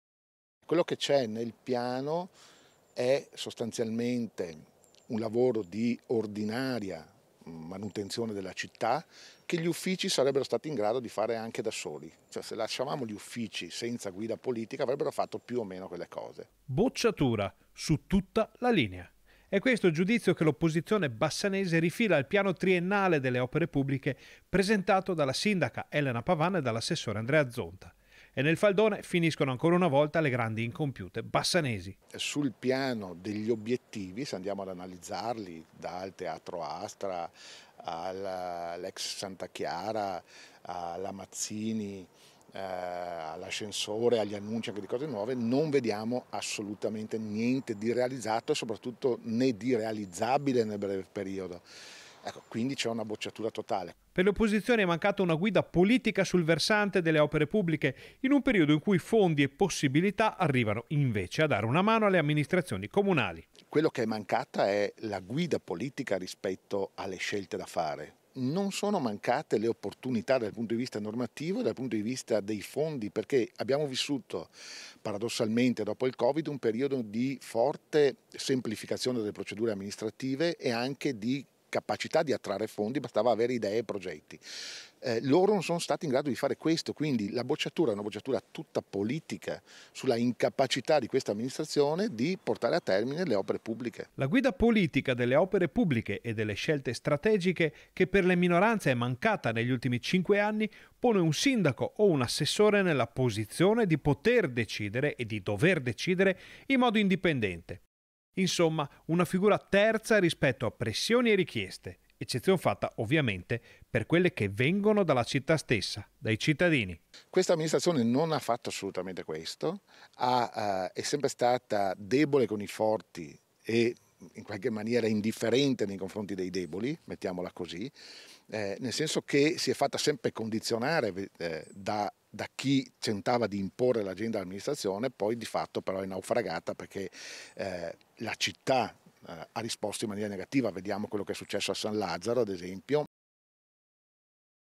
OPERE PUBBLICHE, OPPOSIZIONI SULLE BARRICATE: «BOCCIATURA TOTALE» | 02/12/2023
TG Bassano del Grappa 02/12/2023 - BASSANO DEL GRAPPA - Dopo la presentazione del piano triennale delle opere pubbliche da parte dell'Amministrazione comunale di Bassano arriva la bocciatura delle opposizioni || Bocciatura su tutta la linea. È questo il giudizio che l’opposizione bassanese rifila al piano triennale delle opere pubbliche presentato dalla sindaca Elena Pavan e dall’assessore Andrea Zonta. E nel faldone finiscono ancora una volta le grandi incompiute bassanesi. Per le opposizioni è mancata una guida politica sul versante delle opere pubbliche, in un periodo in cui fondi e possibilità arrivano invece a dare una mano alle Amministrazioni comunali. La guida politica delle opere pubbliche e delle scelte strategiche, che per le minoranze è mancata negli ultimi cinque anni, pone un sindaco o un assessore nella posizione di poter decidere e dover decidere in modo indipendente. Insomma, una figura terza rispetto a pressioni e richieste. Eccezion fatta, ovviamente, per quelle che vengono dalla città stessa, dai cittadini. - Intervistati ROBERTO CAMPAGNOLO (Consigliere Comunale) (Servizio di Ferdinando Garavello) - Segui Rete Veneta anche sul digitale terrestre! Visita il sito